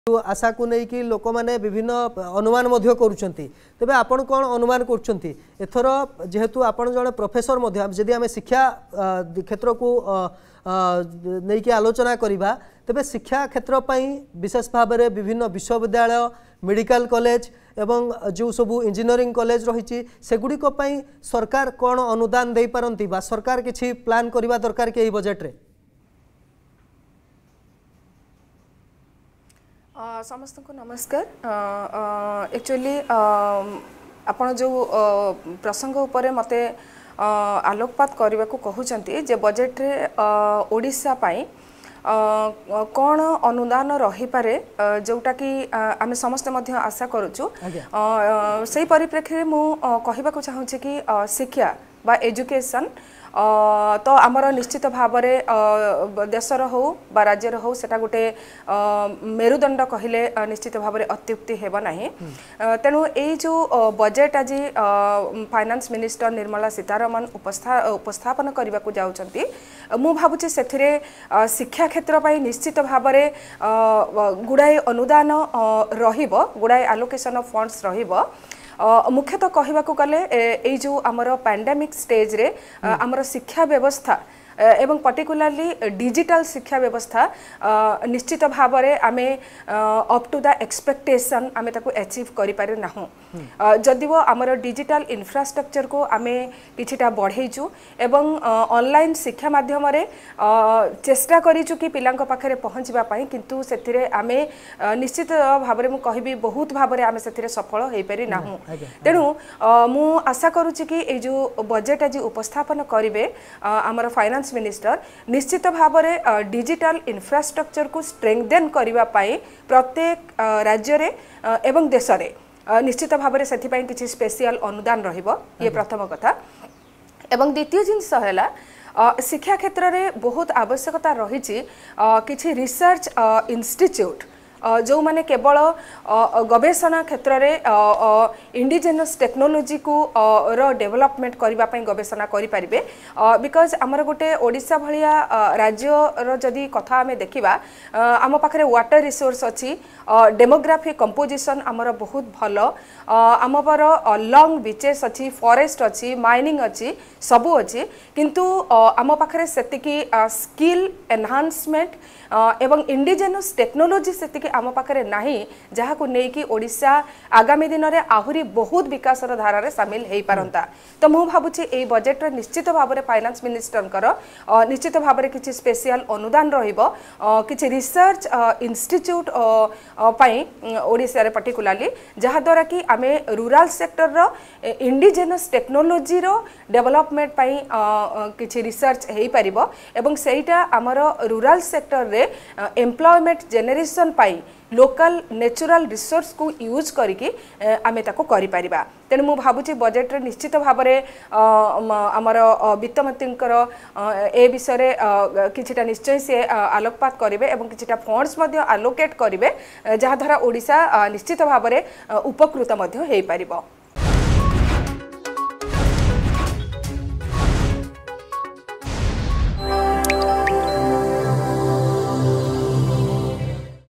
Union Budget 2022: Education experts Expect better Digital Infrastructure in Budget 2022
Education experts Expect better digital infrastructure in Budget 2022 as education is the backbone of our nation.